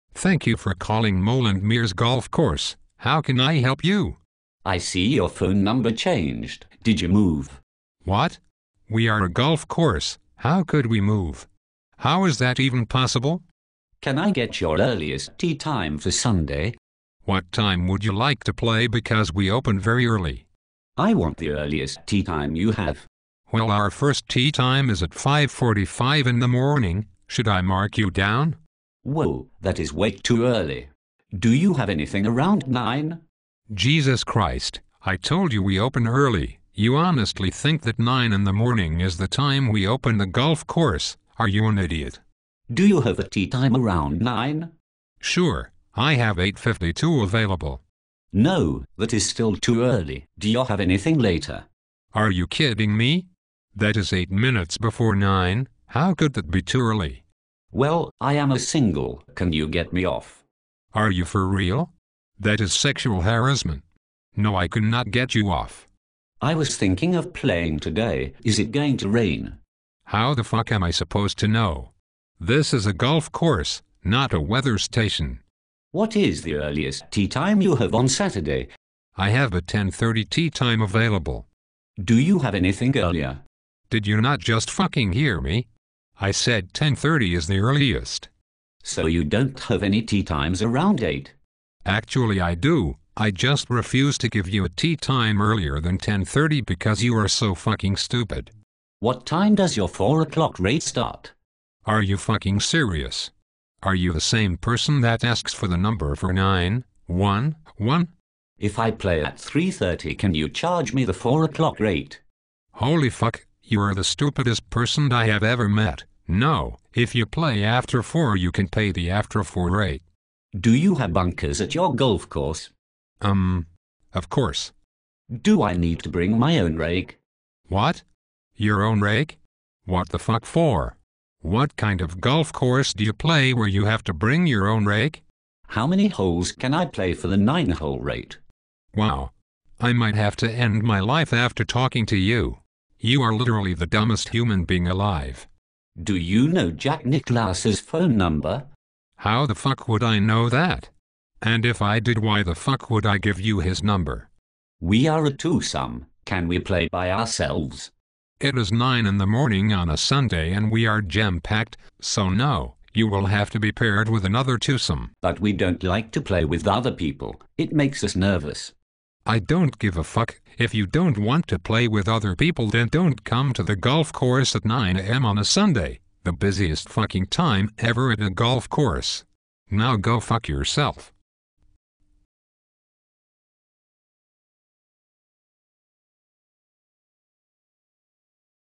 A Day Working in a Golf Shop
***Become our fan on facebook, Mole & Meares both used to work in a golf shop.  These are real questions that either one or both of us have gotten on numerous occasions.  Golf might be a gentleman's game but it has its share of stupid people too!